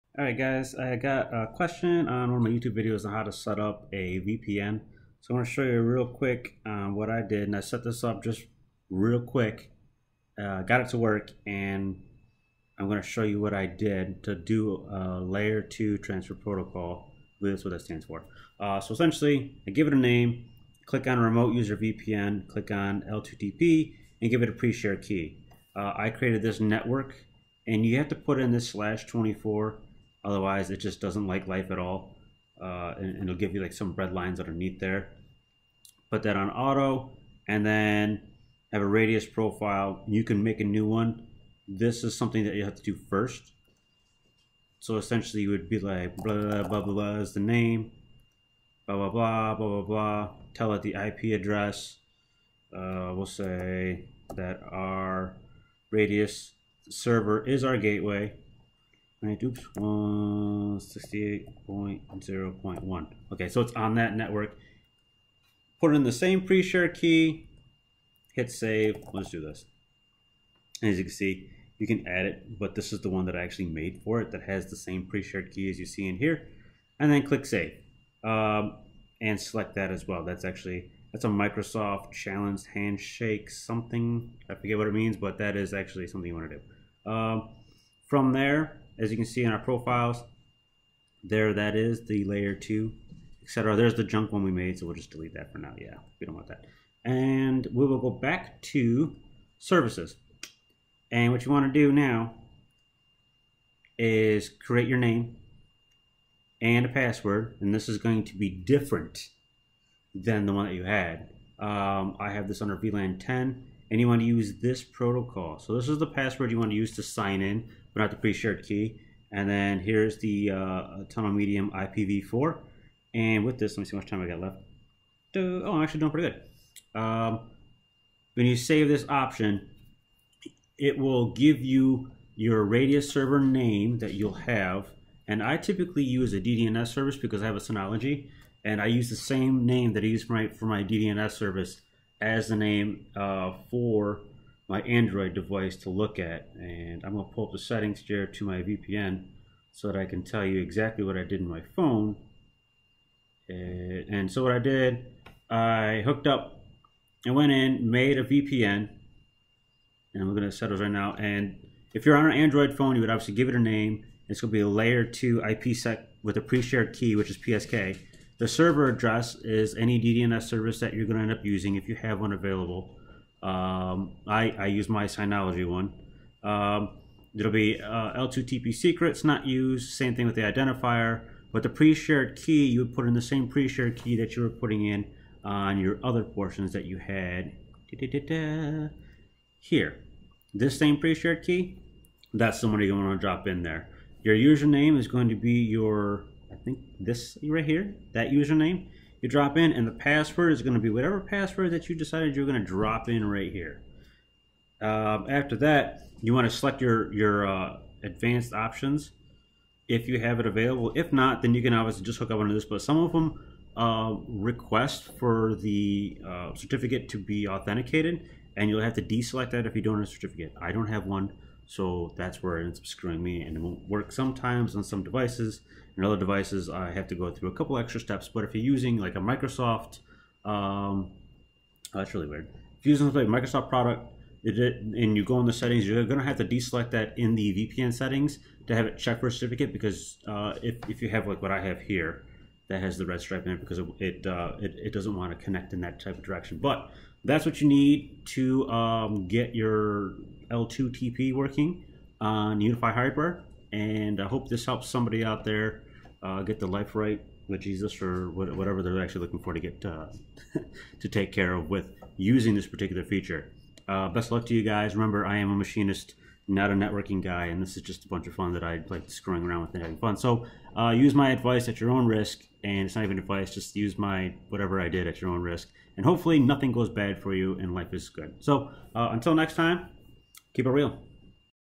Unifi USG L2TP VPN Setup
Quick "how to" on setting up your L2TP VPN using your Unifi USG. Remember, I am not a professional, I am Just doing the best I can.